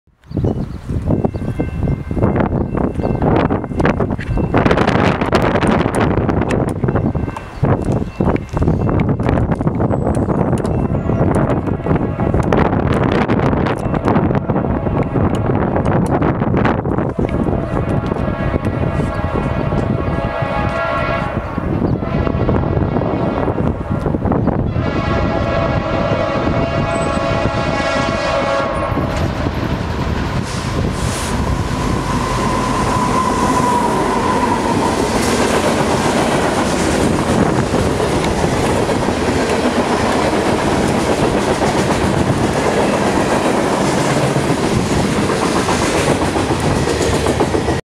NS H44 18
Norfolk Southern H44, returning to Allentown, after working Abrams yard.